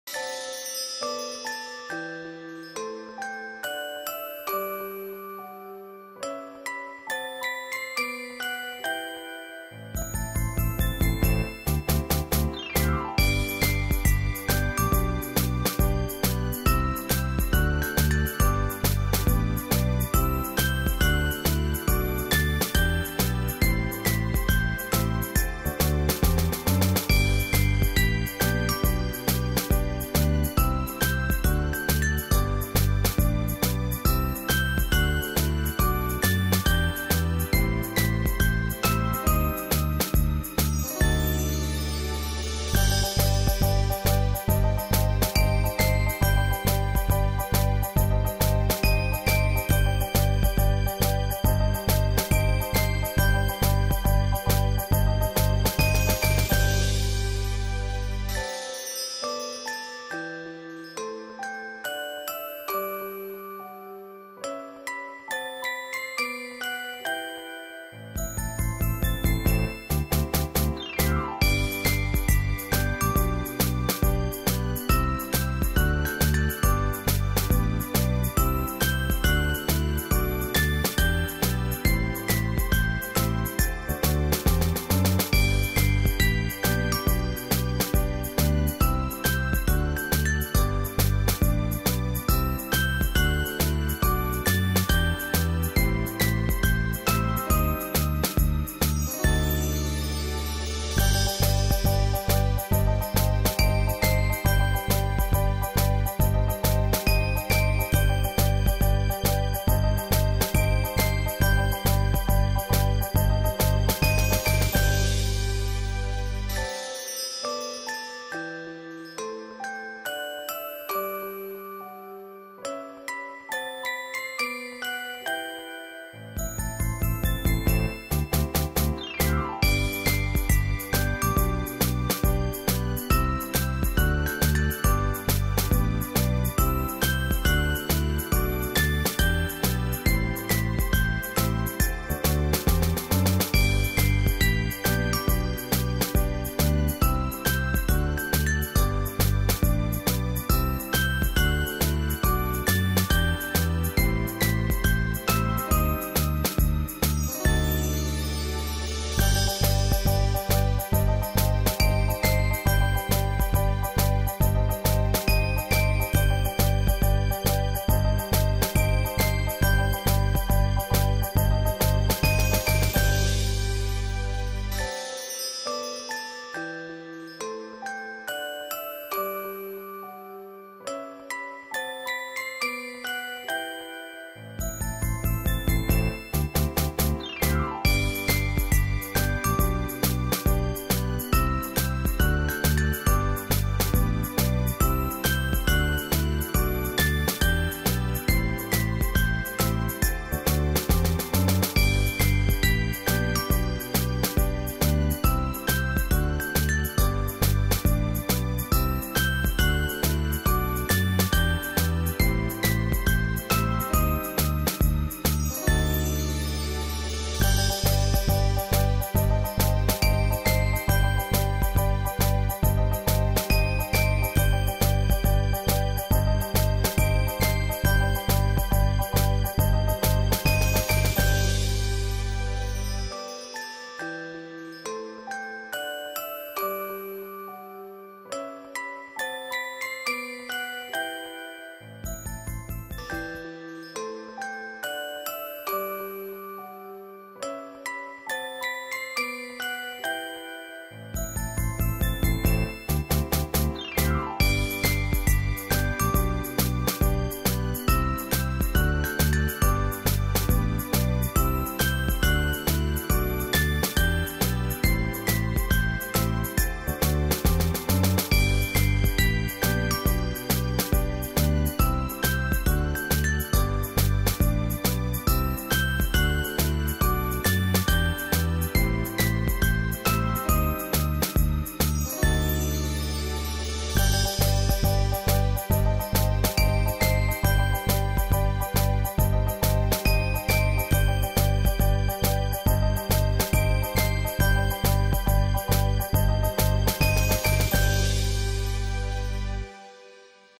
How to make Christmas tree decorations toys.
In this video will show you all, How to make Christmas tree decorations toys.